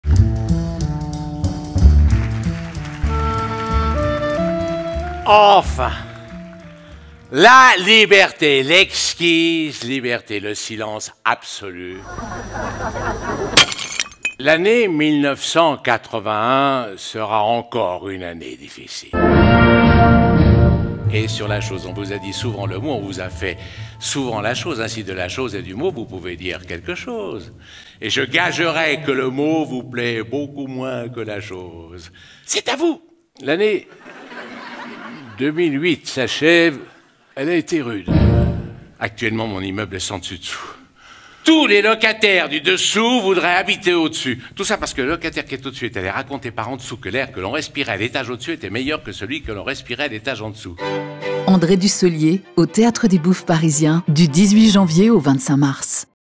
Bande-annonce André Dussollier - Extraits SENS DESSUS DESSOUS
Jusqu'au 25 mars 2023.

Il y a plus de 20 ans à présent, André Dussollier créait son Monstres sacrés, sacrés monstres, patchwork de textes des plus illustres écrivains sans imaginer qu’il deviendrait lui-même, aujourd’hui, ce monstre sacré, voire ce sacré monstre ! Virtuose de la langue, magicien des mots, orfèvre du phrasé, interprète inventif à l’infini, conteur hors pair, André Dussollier est devenu ce sacré monstre de la scène (et de l’écran !), un artiste rare et exceptionnel ayant placé l’exigence et la finesse au sommet de son engagement.

De sa voix délicieusement magnétique, de son regard perçant d’acuité, de son expressivité hors du commun, il nous charme et nous ensorcelle pour nous entrainer dans son imaginaire sans limite en nous révélant avec gourmandise ce qui constitue la substantifique moëlle des grands textes ou la fulgurance des grands esprits. C’est un véritable bonheur que de retrouver dans le cocon du Théâtre des Bouffes Parisiens cet artiste d’exception dans ce voyage sens dessus dessous dans les méandres de la littérature et de la féérie…

André Dussollier

Une pièce conçue et réalisée par André Dussollier
Durée du spectacle : 1h30